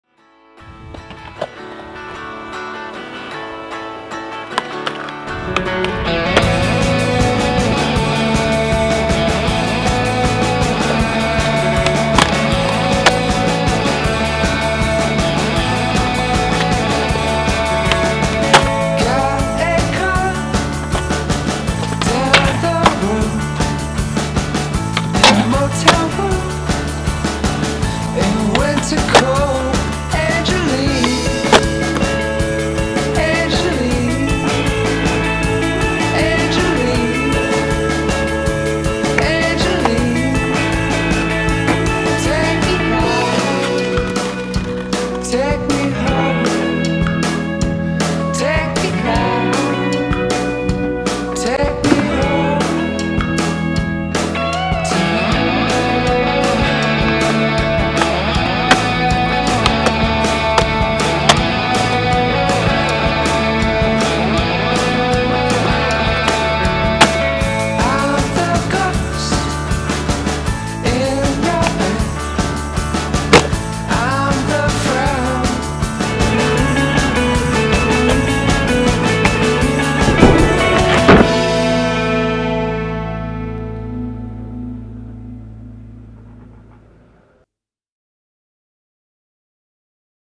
Justin Laugen skates Benicia X Park
Wheel in Motion team rider, Justin Laugen, skated the local skatepark today for a few hours and was able to put together this little edit.